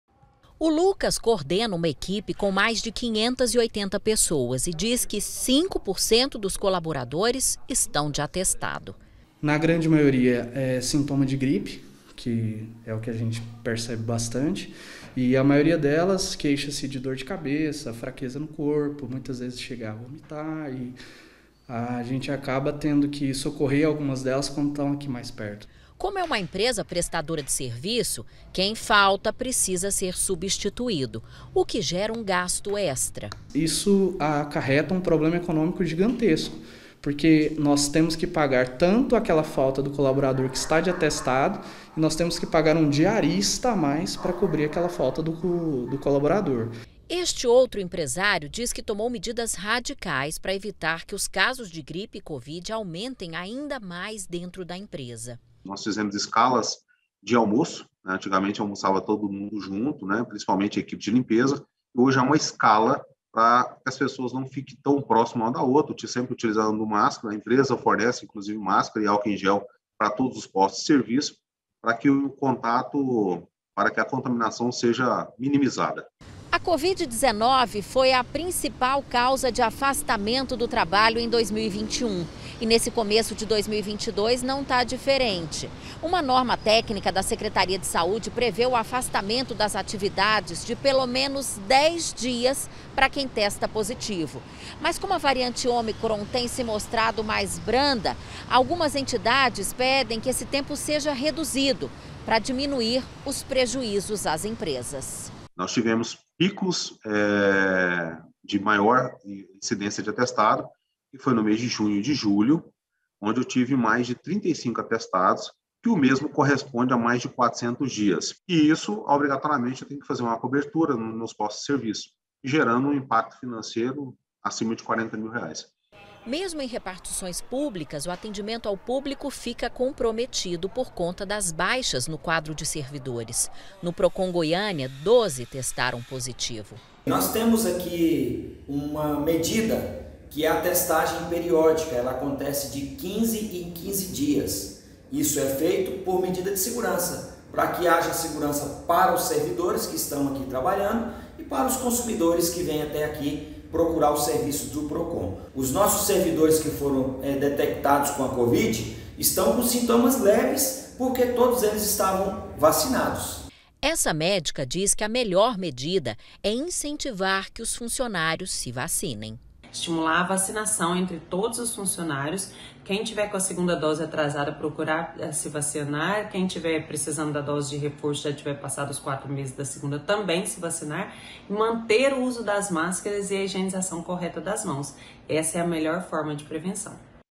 GRIPE: FUNCIONÁRIOS AFASTADOS E PREJUÍZO PARA EMPRESAS
A GRIPE TAMBÉM PREOCUPA PELA FACILIDADE DE CONTÁGIO E PELOS SINTOMAS MUITO PARECIDOS COM OS DE OUTRAS DOENÇAS. E NÃO É SÓ DO NOSSO CORPO QUE ELA JUDIA.